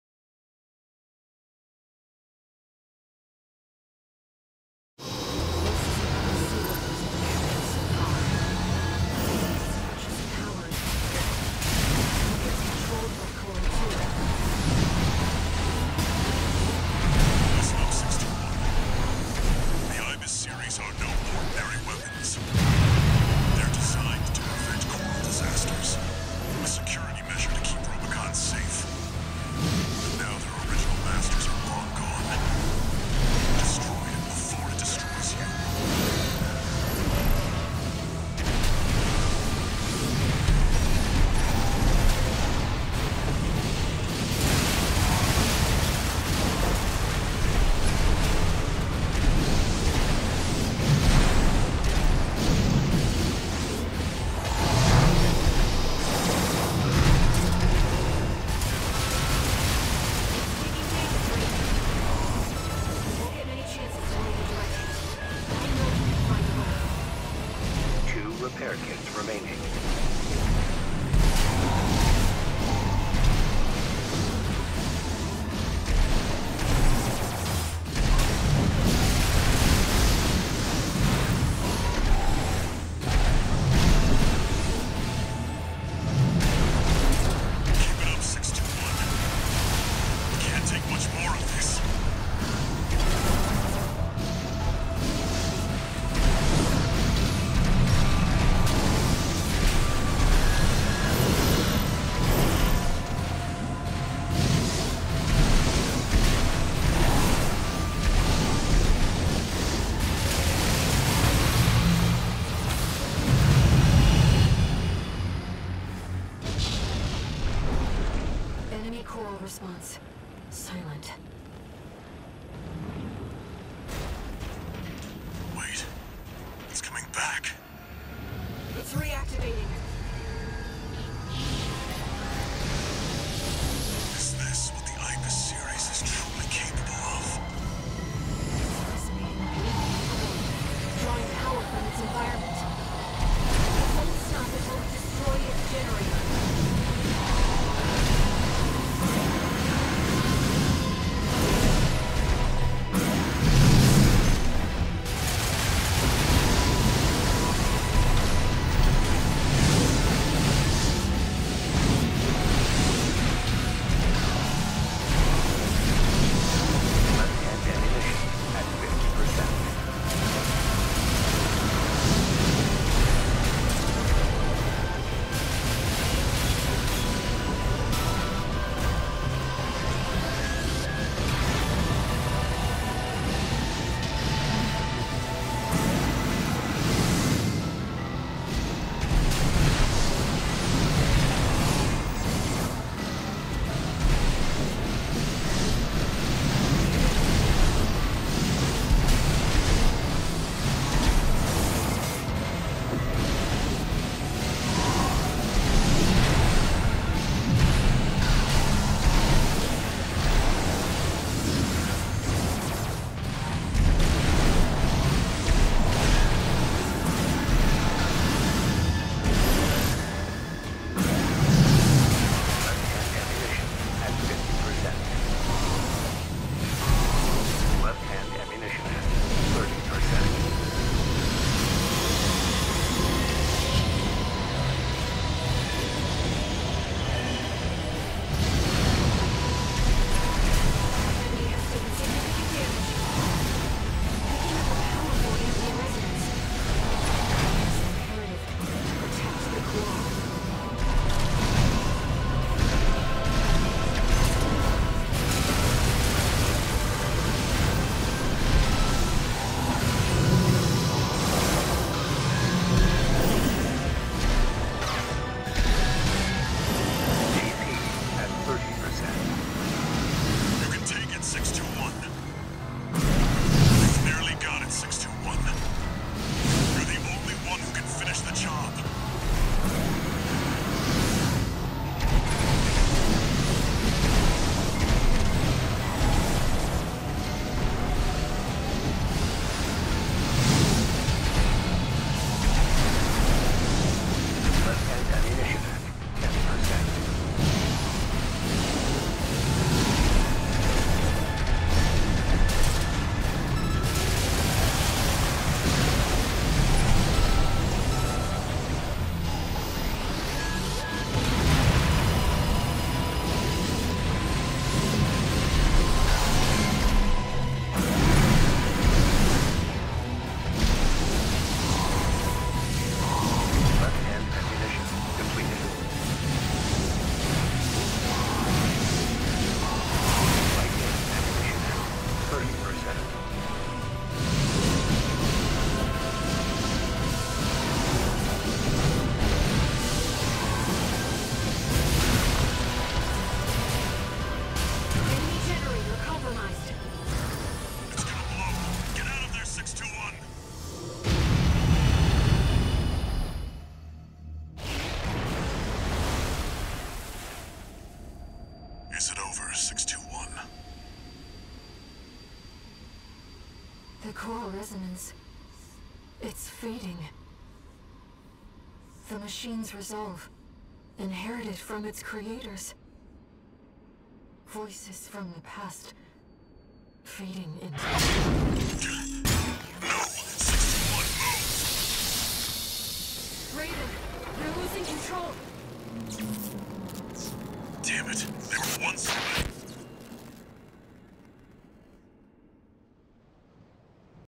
빠른놈 vs 빠른놈
아이비스 공격을 순수 기체 스펙으로 피하는 지경!!